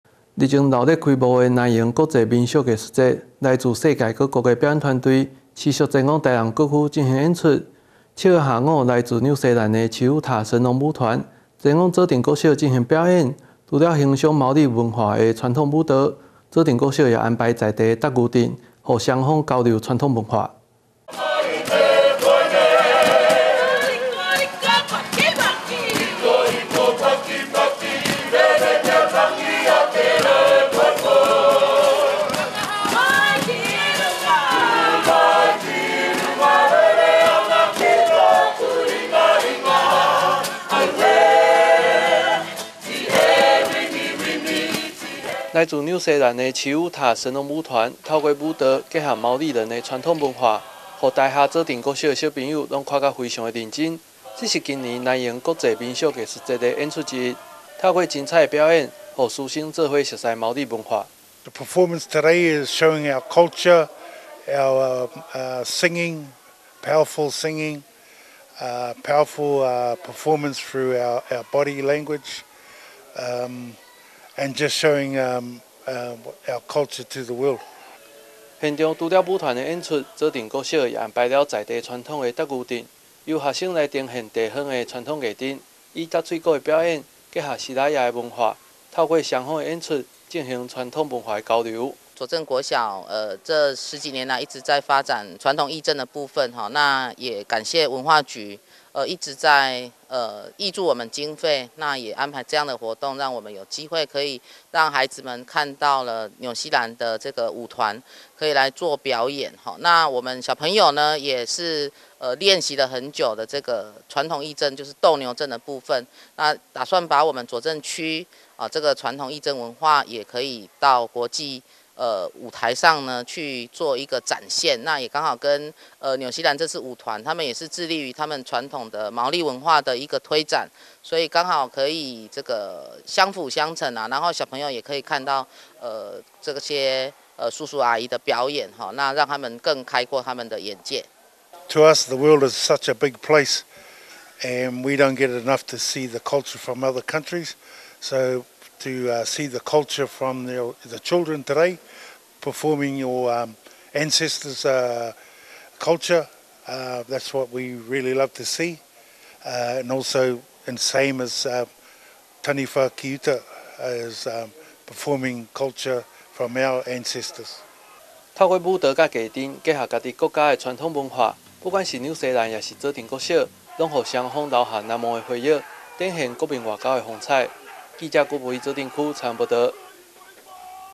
左小與紐西蘭舞團國際交流 認識雙方文化-新永安新聞
日前熱鬧開幕的南瀛國際民俗藝術節 來自世界各國的表演團隊 持續前往臺南各區進行演出 7日下午來自紐西蘭的奇巫塔神龍舞團 前往左鎮國小進行演出 除了欣賞毛利文化的傳統舞蹈 左鎮國小也安排在地的鬥牛陣 讓雙方交流傳統文化